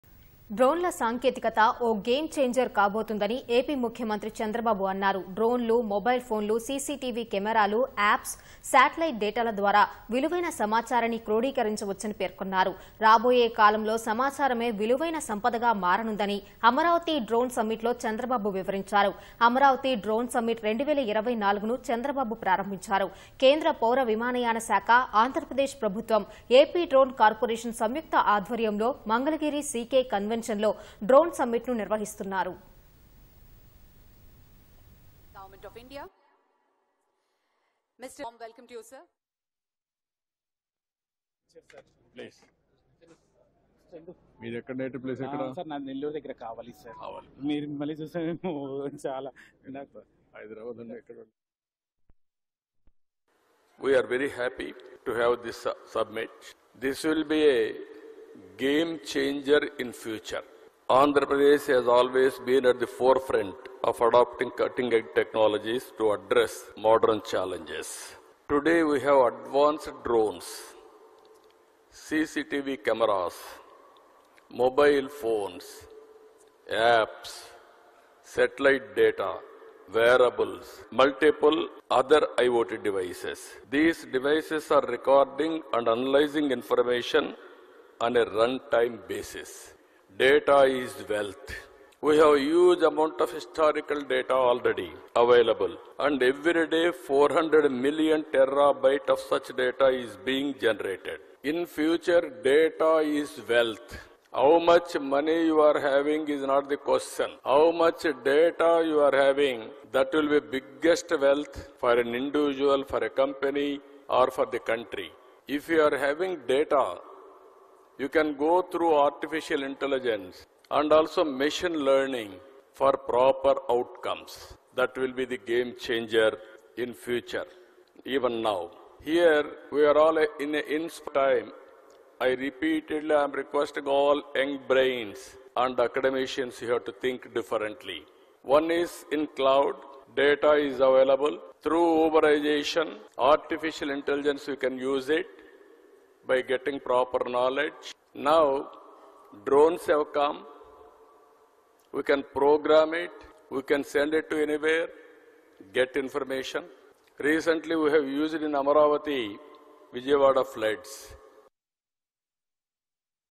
CM Chandrababu Inaugurated Amaravati Drone Summit | అమరావతి డ్రోన్ సమ్మిట్‌ను ప్రారంభించిన చంద్రబాబు
డ్రోన్ల సాంకేతికత... ఓ గేమ్ ఛేంజర్ కాబోతుందని AP ముఖ్యమంత్రి చంద్రబాబు అన్నారు. డ్రోన్లు, మొబైల్ ఫోన్లు, CCTV కెమెరాలు, యాప్స్, శాటిలైట్ డేటాల ద్వారా... విలువైన సమాచారాన్ని క్రోడీకరించవచ్చని పేర్కొన్నారు. రాబోయే కాలంలో సమాచారమే విలువైన సంపదగా మారనుందని... అమరావతి డ్రోన్  సమ్మిట్ లో చంద్రబాబు వివరించారు. అమరావతి డ్రోన్ సమ్మిట్ -2024 ను చంద్రబాబు ప్రారంభించారు. కేంద్ర పౌరవిమానయాన శాఖ, ఆంధ్రప్రదేశ్  ప్రభుత్వం, AP డ్రోన్ కార్పొరేషన్  సంయుక్త ఆధ్వర్యంలో మంగళగిరి C.K. కన్వెన్షన్ లో డ్రోన్  సమ్మిట్ ను నిర్వహిస్తున్నారు..